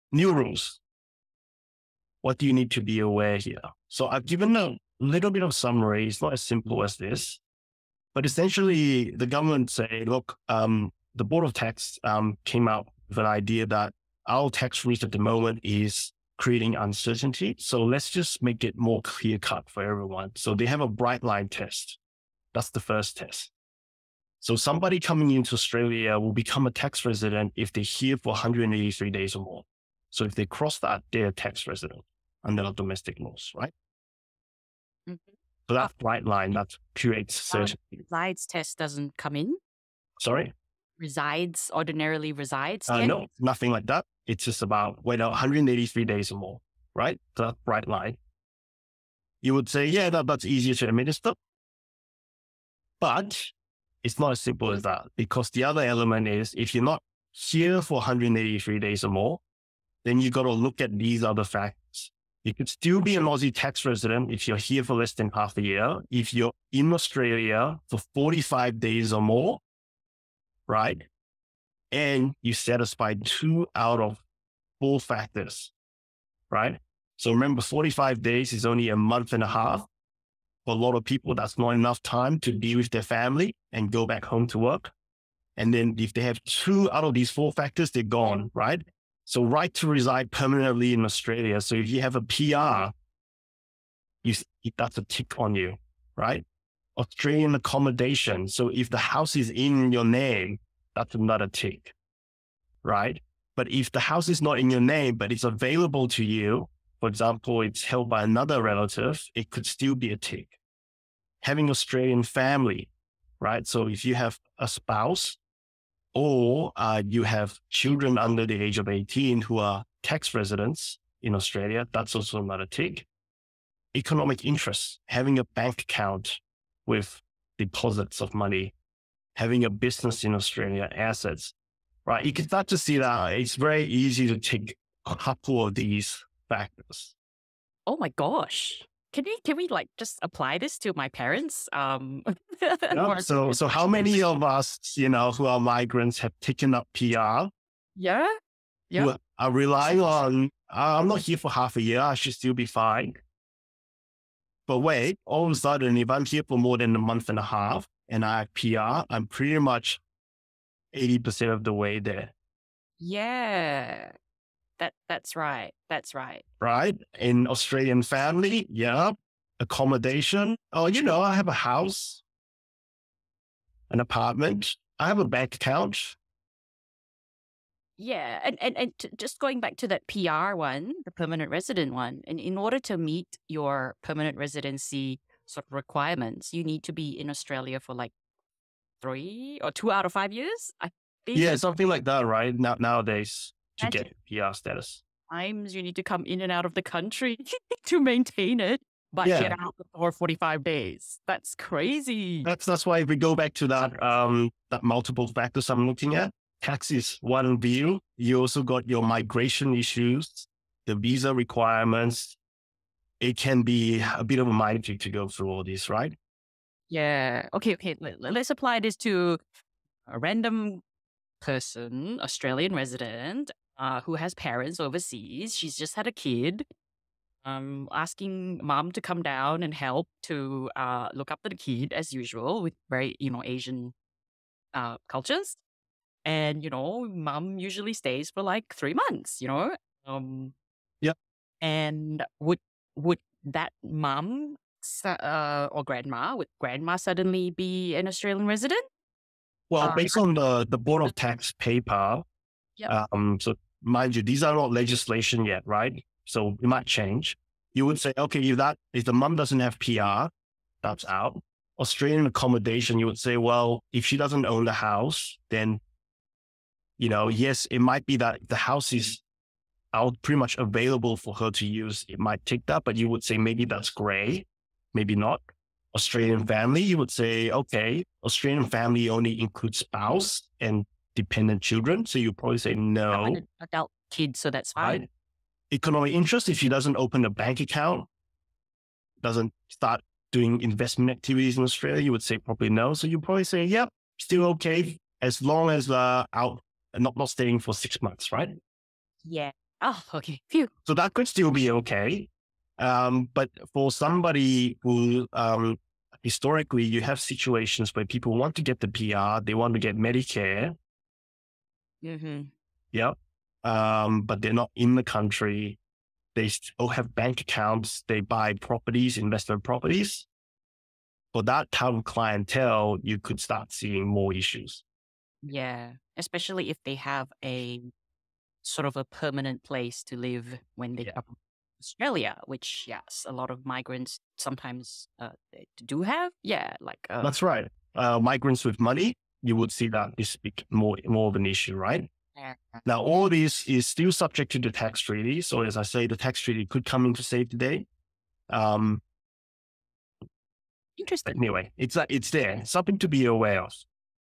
Proposed New Individual Residency Rules
When planning for a migration, don't forget to consider our proposed individual residency rules! These rules are significantly different from the current rules, particularly for inbound travellers, that is, for foreign residents intending to stay in Australia for a period of time.

Under the proposed rules, Australian tax residency may be triggered as soon as a person is in Australia for 45 days or more. Yikes!😓

King Tan of Coleman Greig breaks this down for us in our latest Tax Nugget on Migration & Taxation.